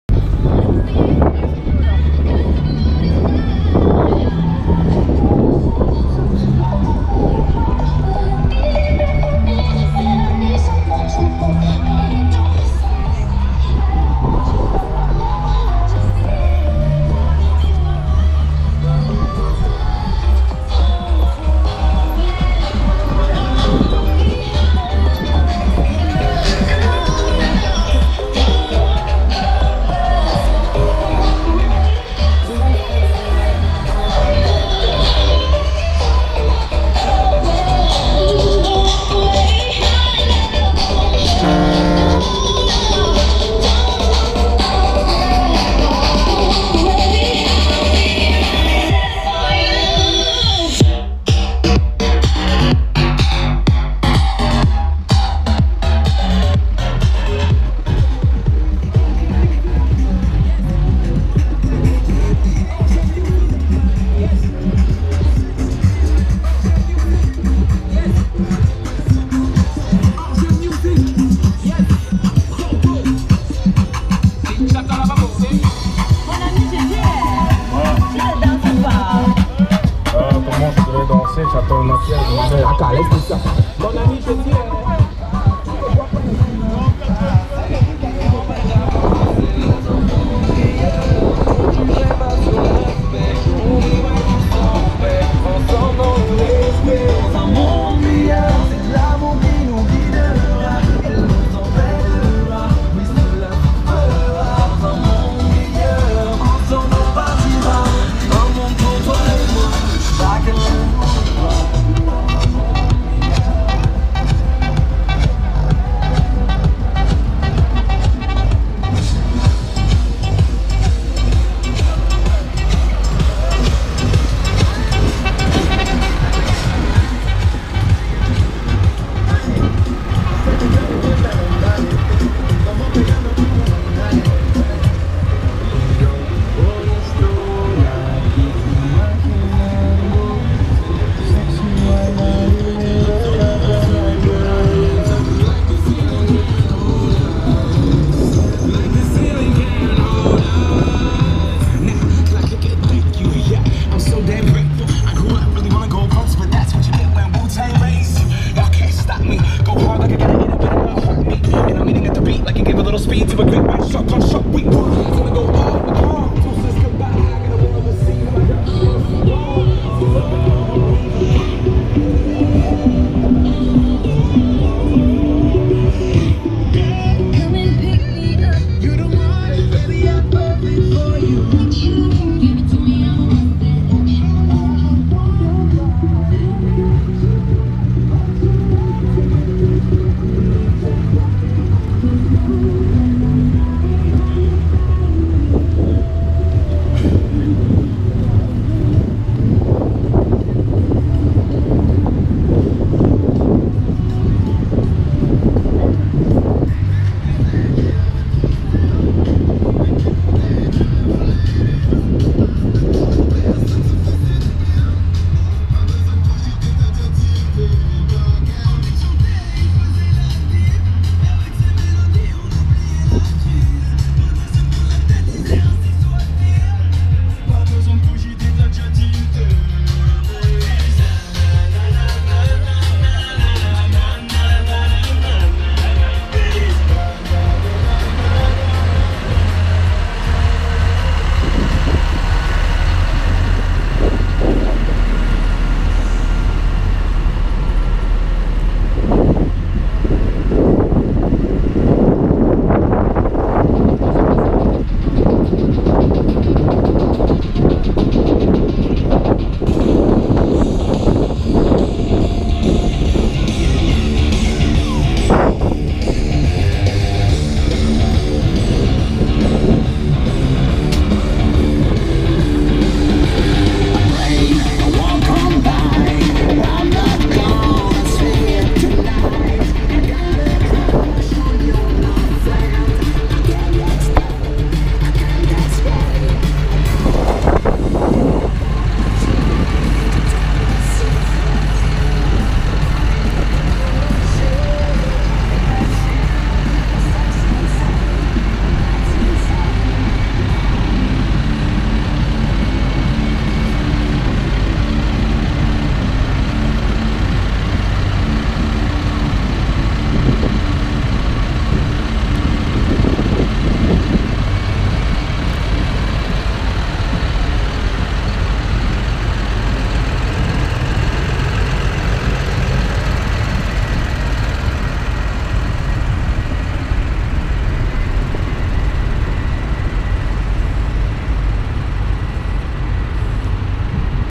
Carnaval Cholet 2016
En route pour le plus grand défilé lumineux de France (voire d'Europe)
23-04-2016